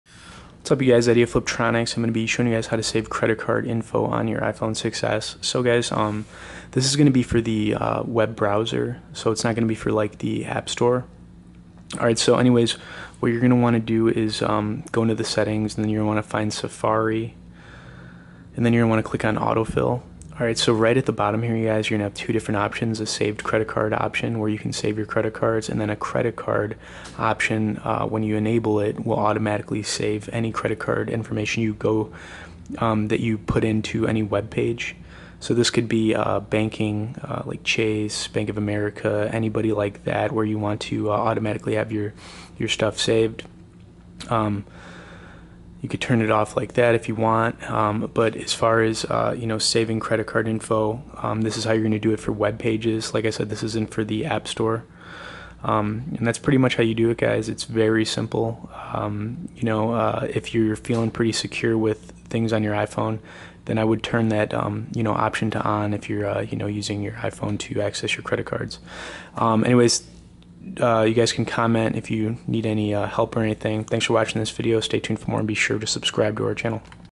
Iphone 6s - How To Save Credit Card - Fliptroniks.com
If on your iphone 6s you need to know how to save your credit card we are going to show you how to do that for the web browser. Your going to want to first go into the settings and then click on safari. You will then see autofill click on it. Once you click on it there will be an option for auto saving credit card info just slide it to on and you will be all set.